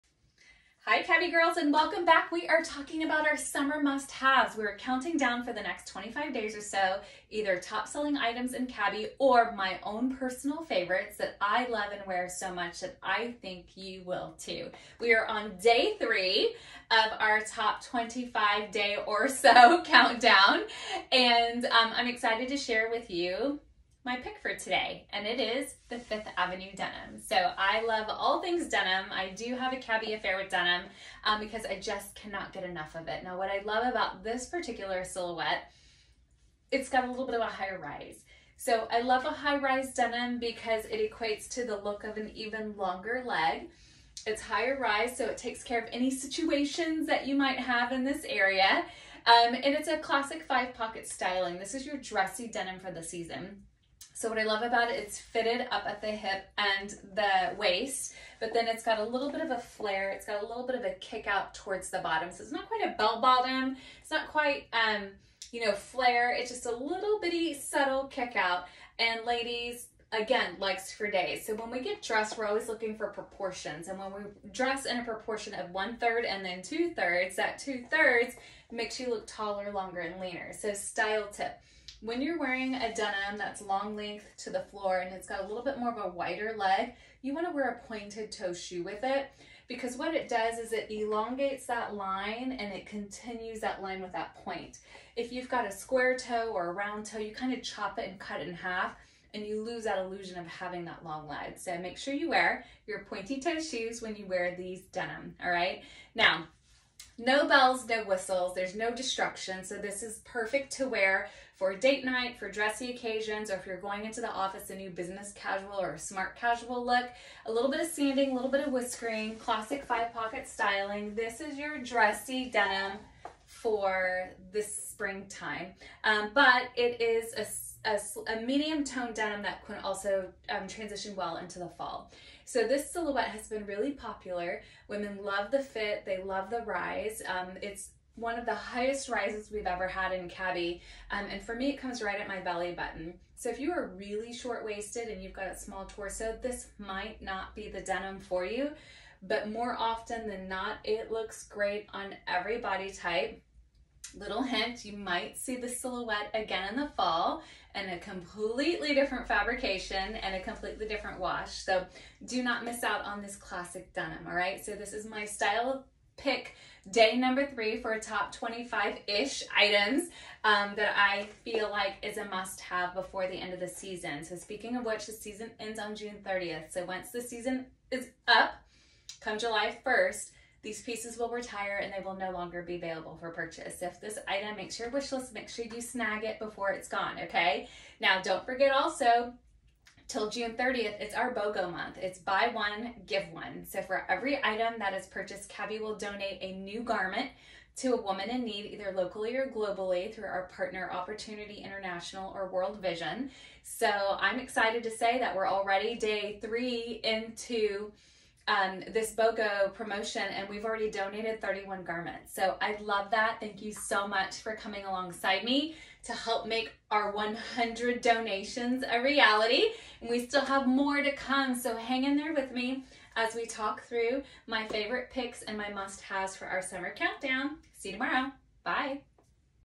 Summer Countdown day 3: 5th Avenue Jean
25 days of my cabi favorites-  grab ’em before they are gone forever!

June 1-30th is BOGO (Buy One, Give One)- for every item purchased, cabi will donate 1 garment to a woman I need!  💪🏻♥️👗